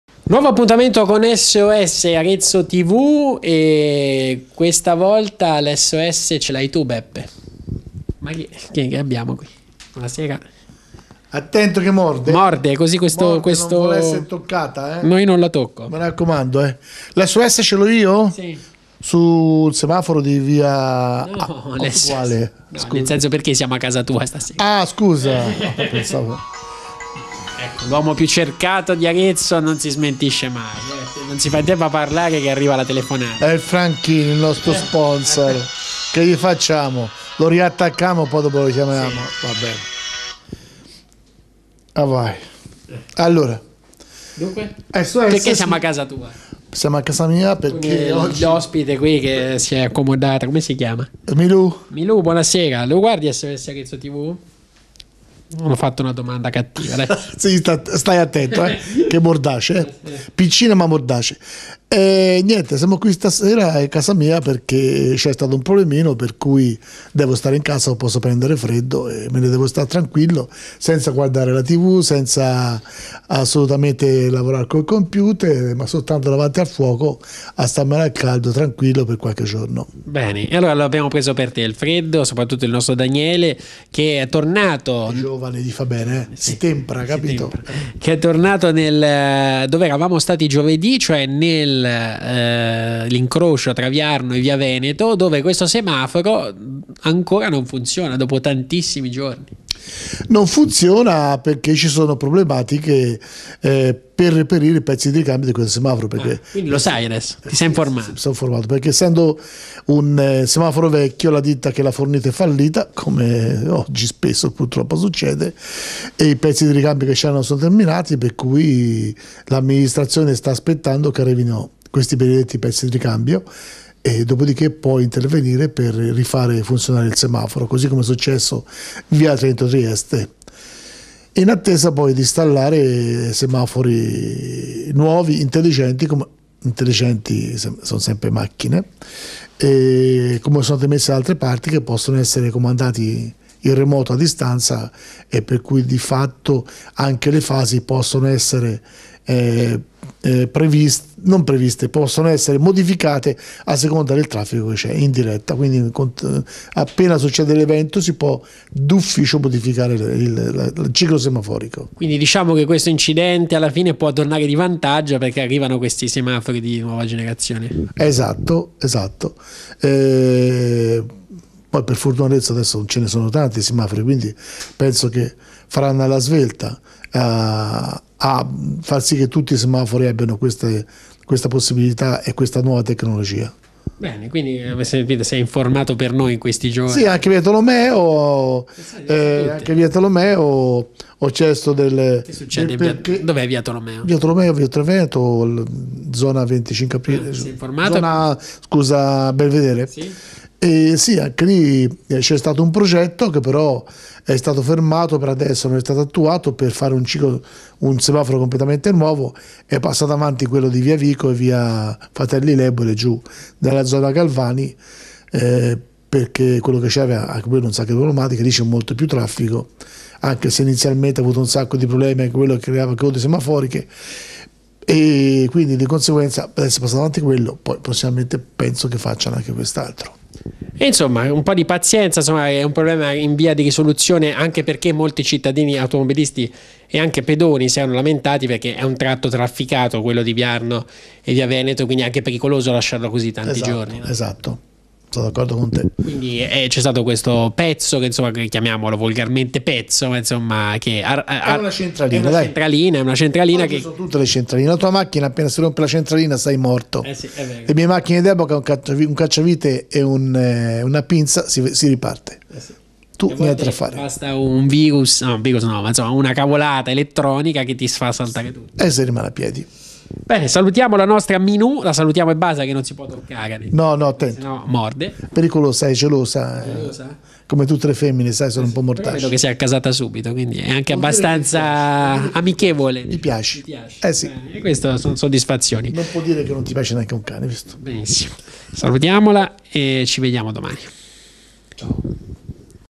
Sos Arezzo Tv: ancora sul semaforo di via Veneto
Ci siamo informati sul perché ancora non è stato ripristinato il semaforo di via Veneto